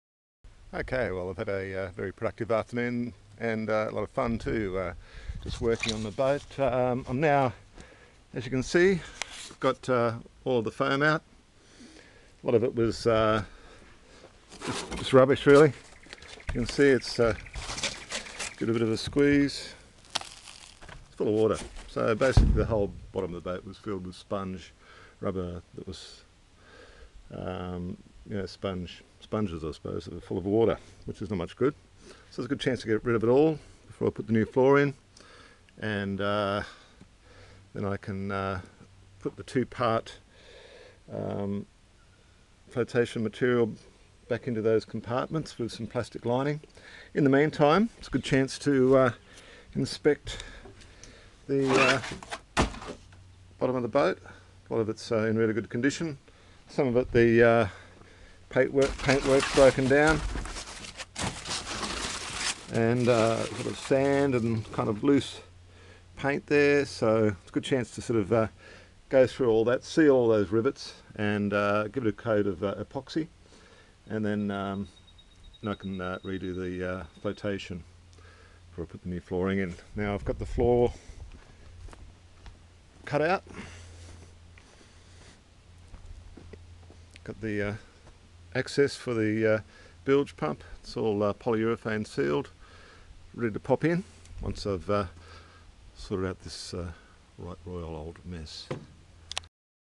replace that rotten floor in your boat DIY part 7
Ive put a coating of polyurathane paint on the new flooring panel which is made from marine ply
What a mess !  Ive dug up the very old flotation foam in between the stringers and its rotten and also totally water logged and I will replace it with new flotation foam ...see part 8 to see how I progress from here